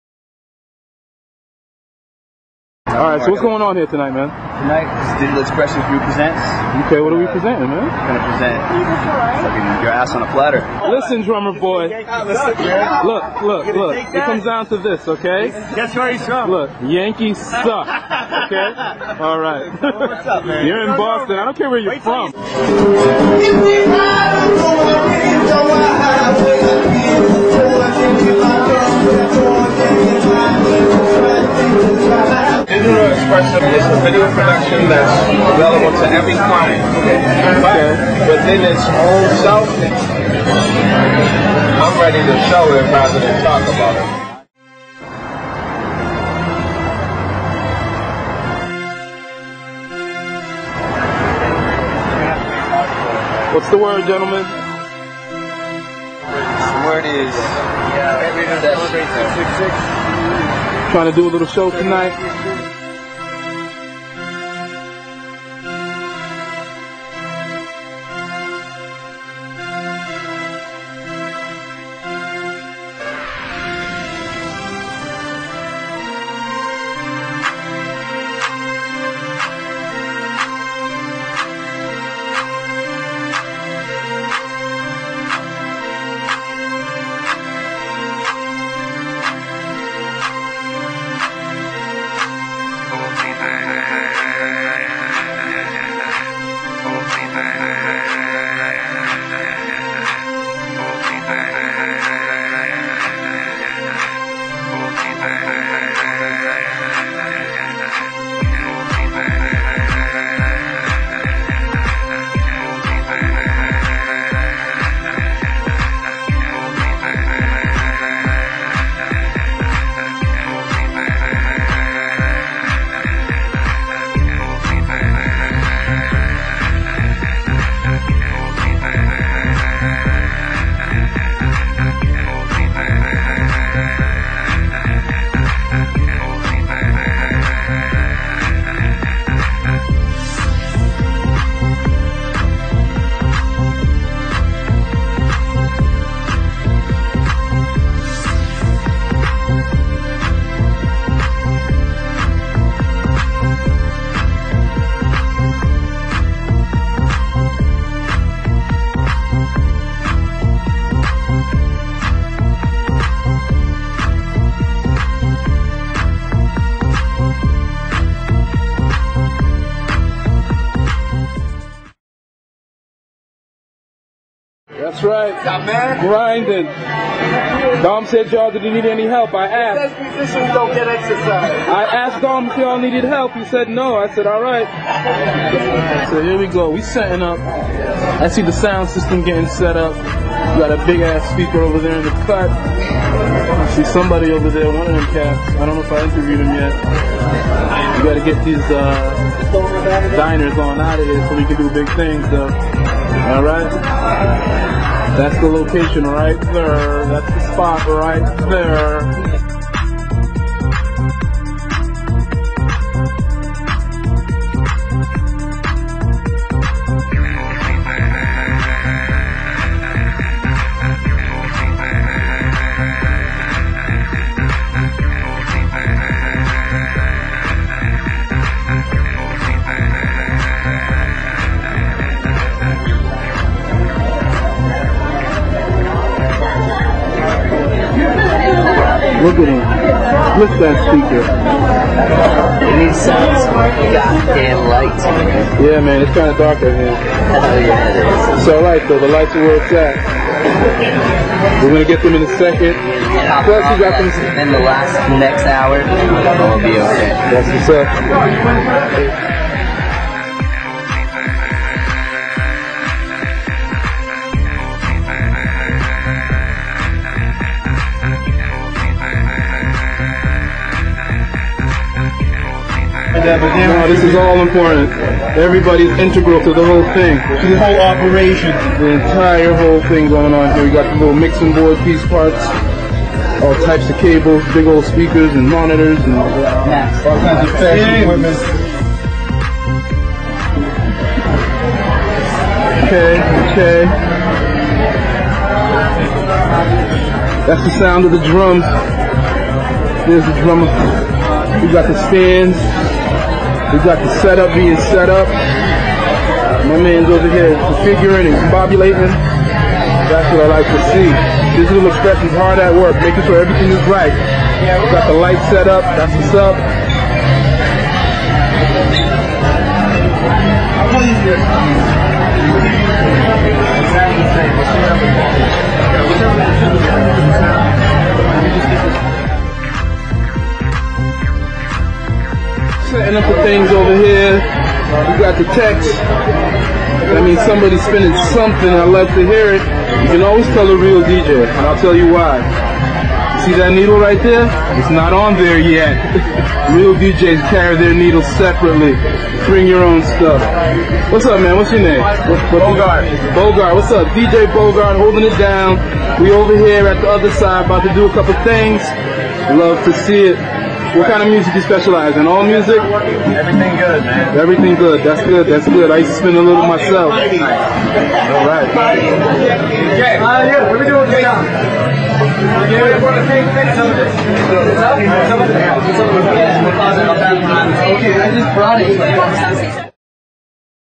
DEG Live! at the Other Side--Part II
Part II, the set up!  check out the behind the scenes footage from the show